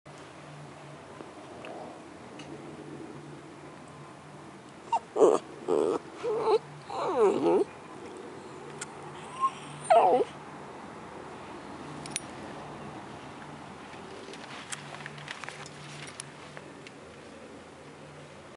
Italian Grey-huahua
My chihuahua / italian greyhound mix being his usual lazy self.